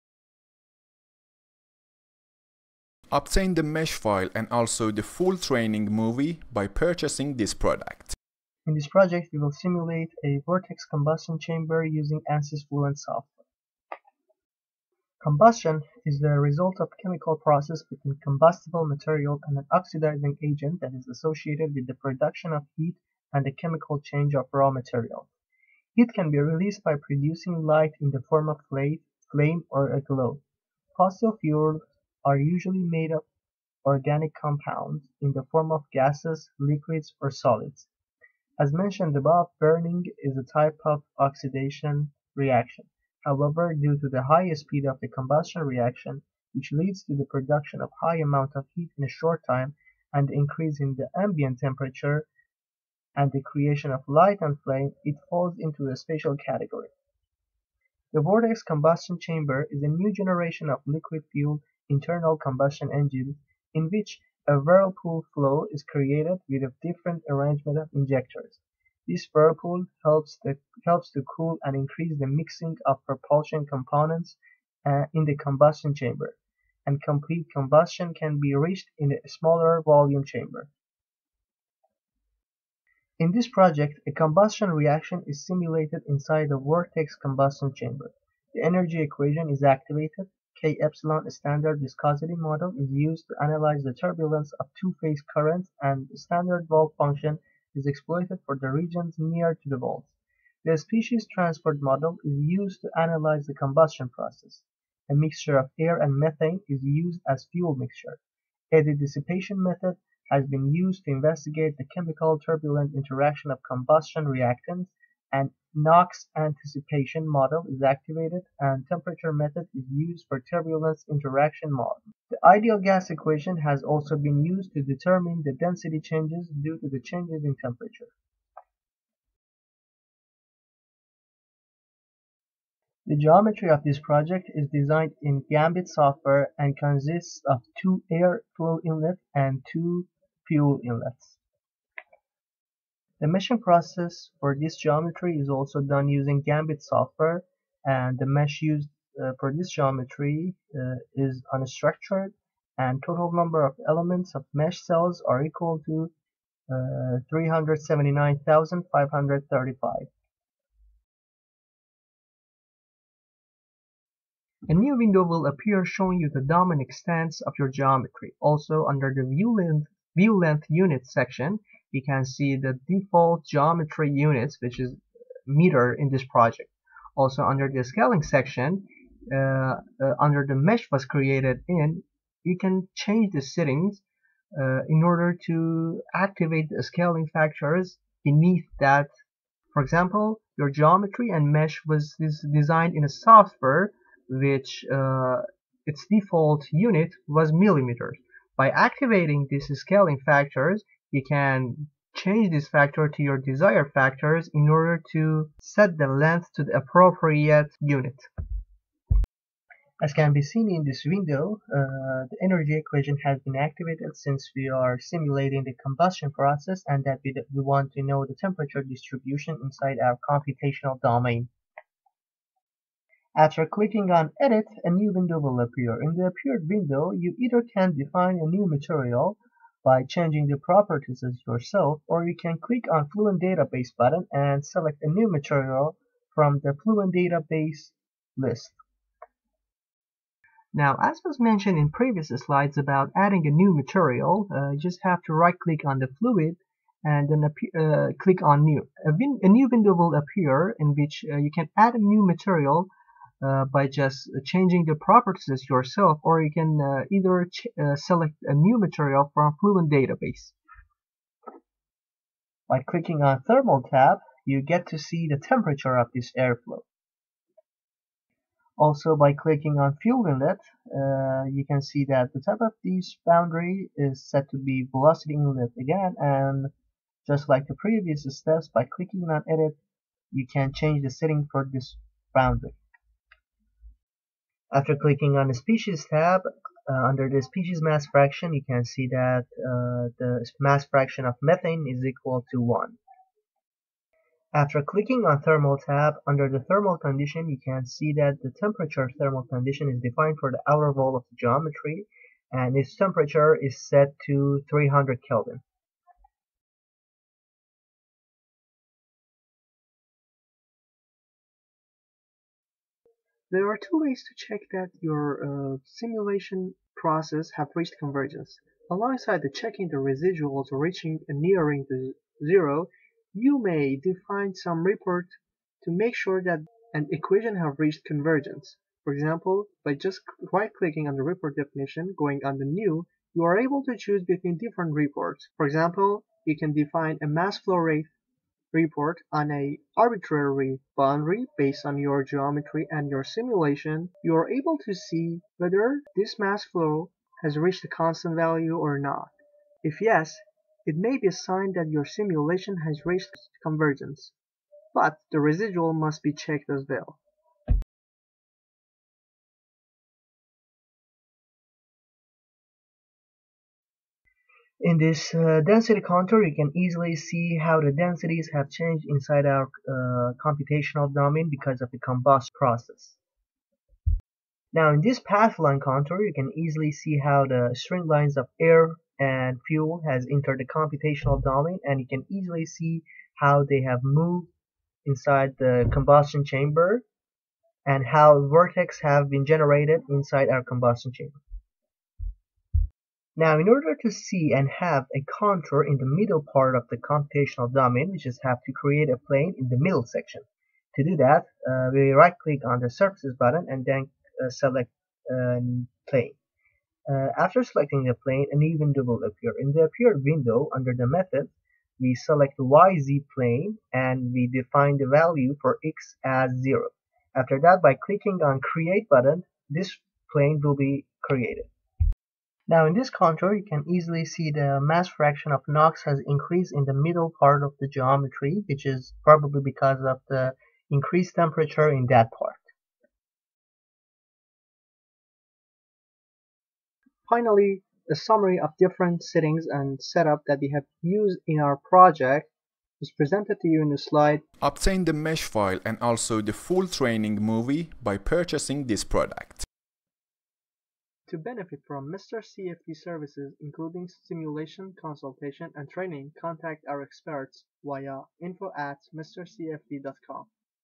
ANSYS FLUENT Training : Vortex Combustion Chamber CFD Simulation
In this project, a combustion reaction is simulated inside a vortex combustion chamber. The energy equation is activated. K-epsilon Standard viscosity model is used to analyze the turbulence of the two-phase current and standard wall function is exploited for the regions near to the walls. The species transport model is used to analyze the combustion process.

A mixture of air and methane is used as the fuel mixture. Eddy-Dissipation method has been used to investigate the chemical-turbulent interaction of combustion reactants and the NOx anticipation model is activated and the Temperature method is used for Turbulence Interaction mode. The ideal gas equation has also been used to determine the density changes due to changes in temperature.